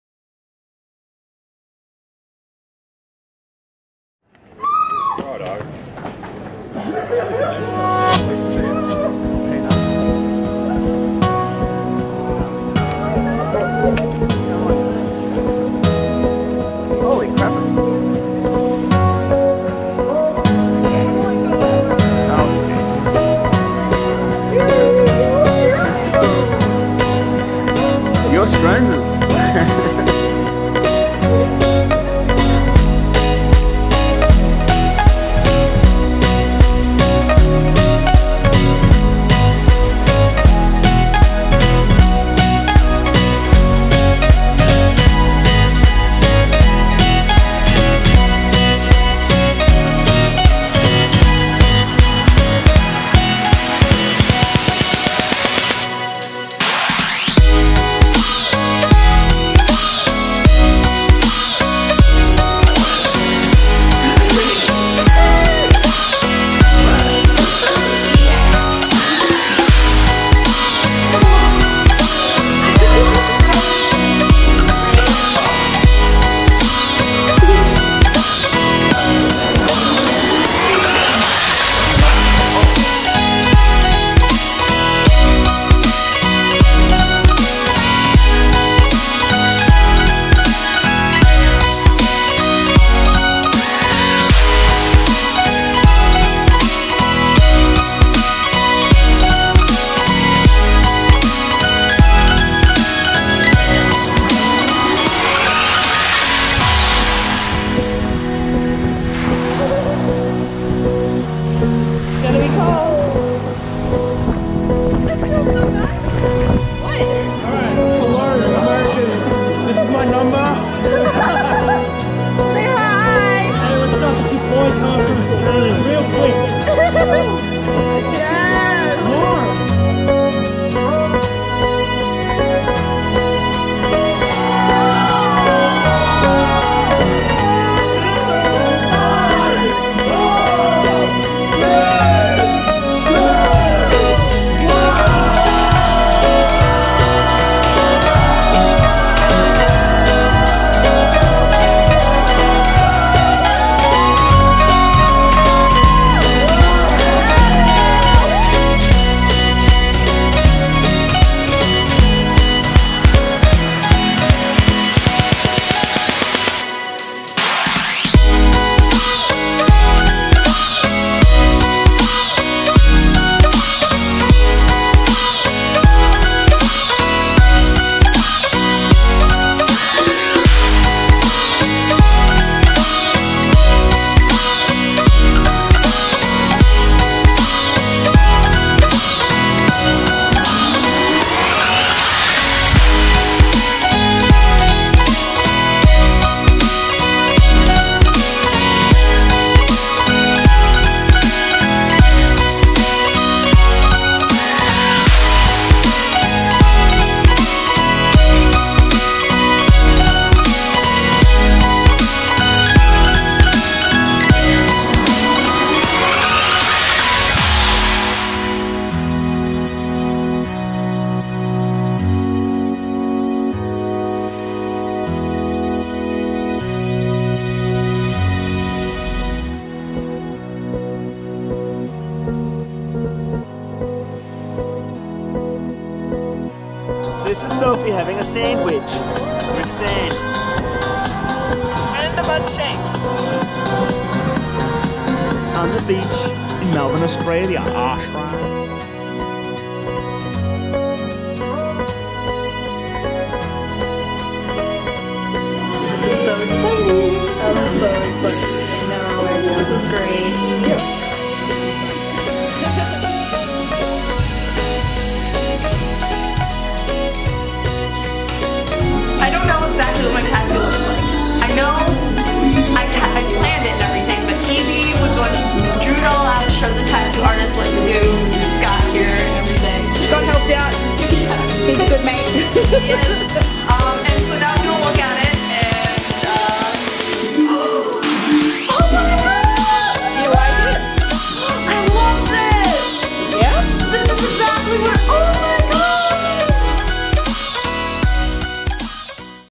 Meeting My Internet BFF of Six Years For The First Time
In December on 2018 I traveled to Melbourne Australia from Green Bay Wisconsin to meet my internet bff for the first time after meeting on Instagram in November of 2012. This is a video for my DSLR Video class with footage from that trip. Enjoy!
-Sophia